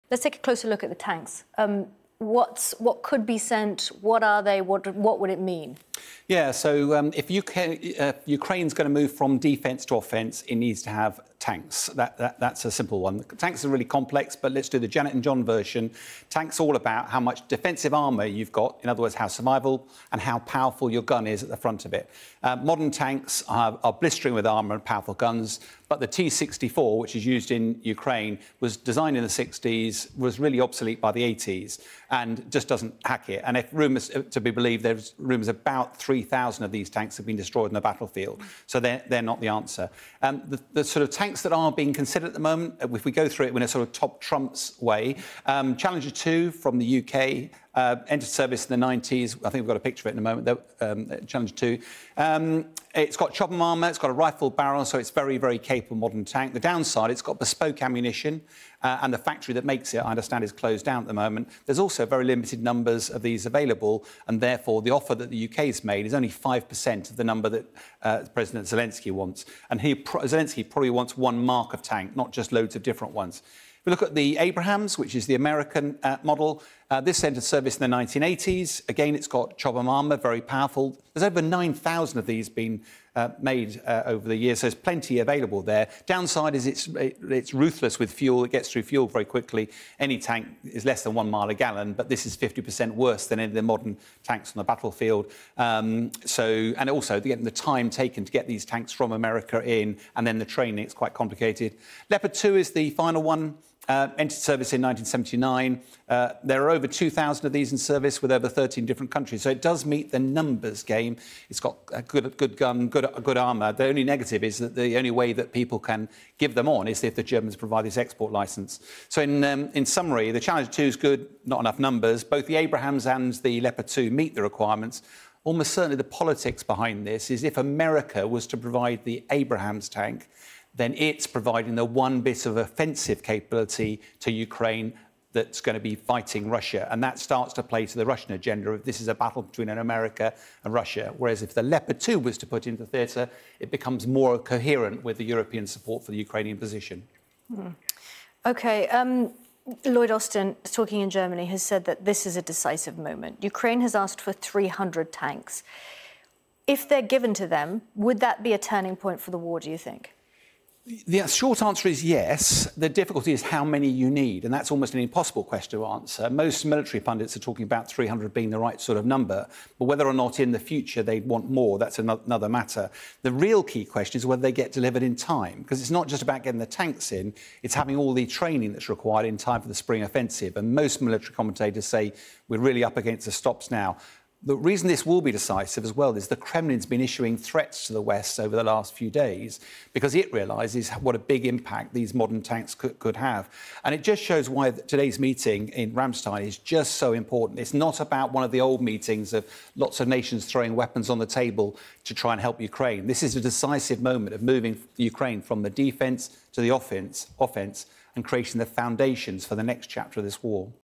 Ukraine War: What tanks could be sent?
Retired Air Vice-Marshal Sean Bell looks at what tanks could be sent to help Ukraine in their offensive with Russia.